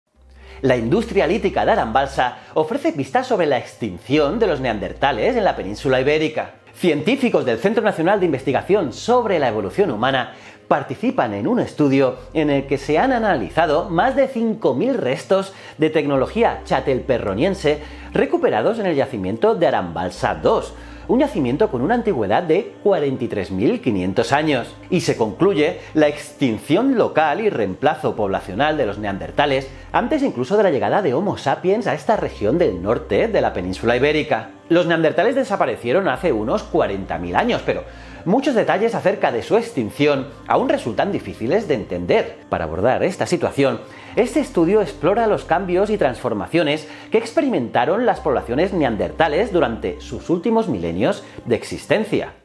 La industria lítica de Aranbaltza ofrece pistas sobre la extinción de los neandertales
La industria lítica de Aranbaltza ofrece pistas sobre la extinción de los neandertales en la península ibérica.